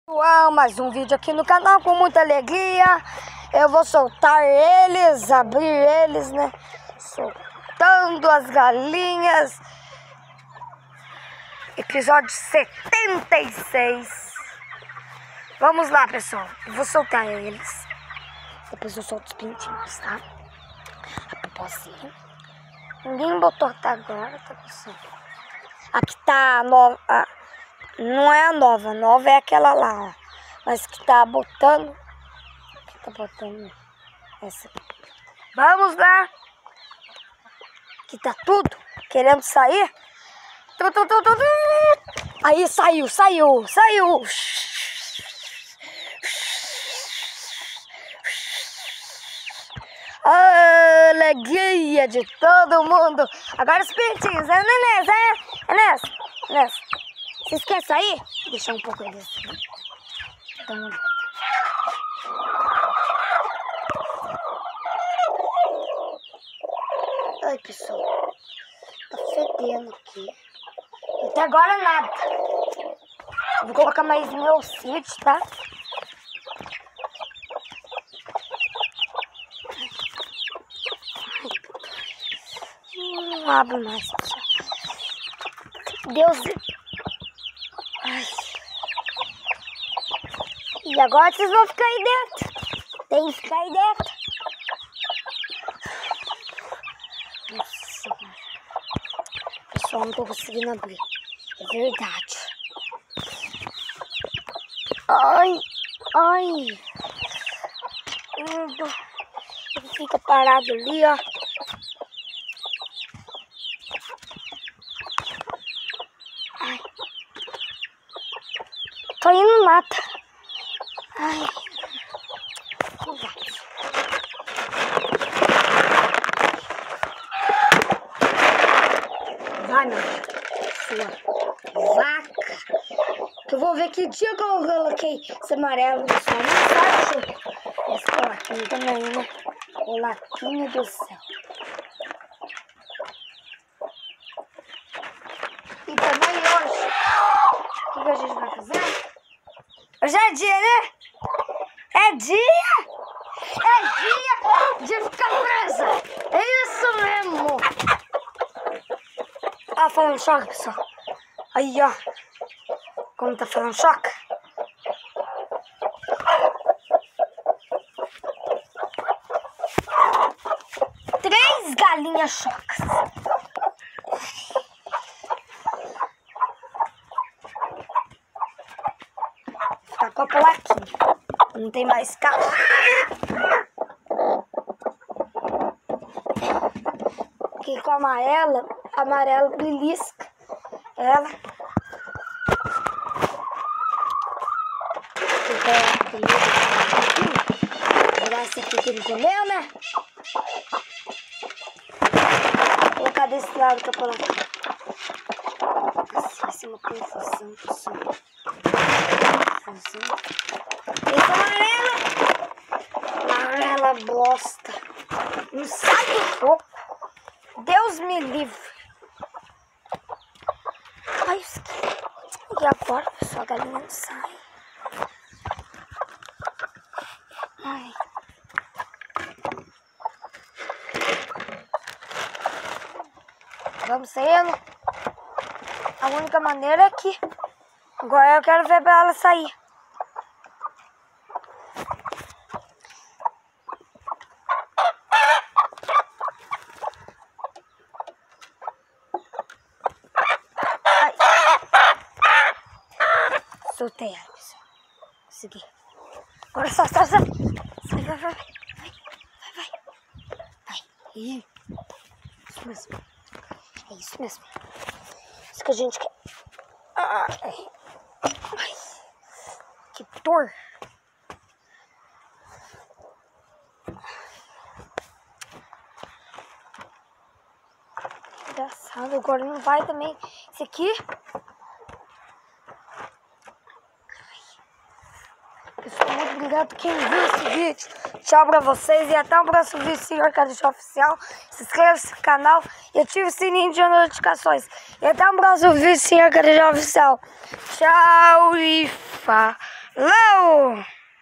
soltando as Galinhas no quintal e alimentando as Galinhas chocas e prendendo uma e falando Ep.76
oi pessoal talvez depois tem mais 1 ou 3 videos então não percam